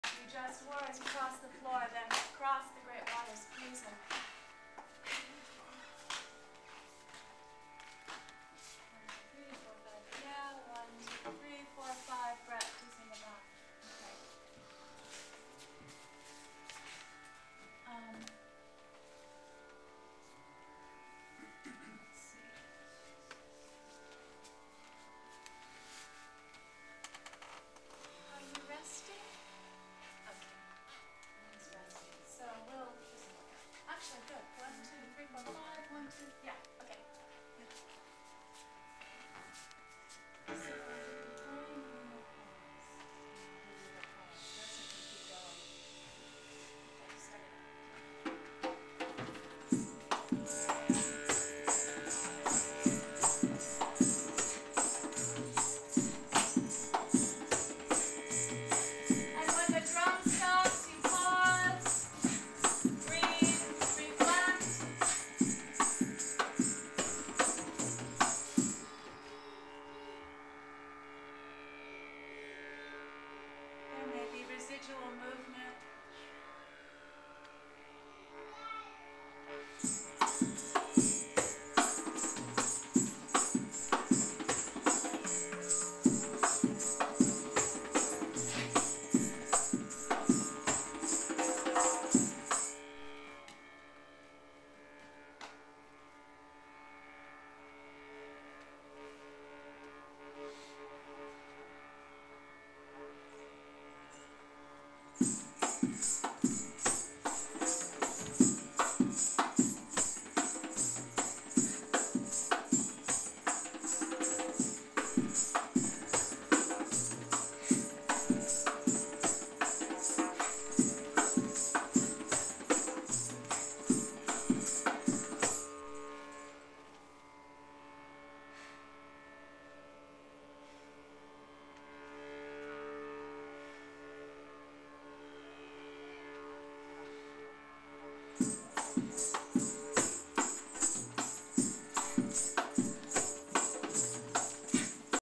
Chaos & Stillness 5/18
Gabrielle Roth's 5Rhythms practice as led by Christine Tasseff on Whidbey Island. Today was a lesson in the dance of paired opposites between wild activity and relaxed stillness in this daily butoh dance by Maureen "momo" Freehill with Christine's voice and musical accompaniment by Joseph Sanchez & Rackman Ross.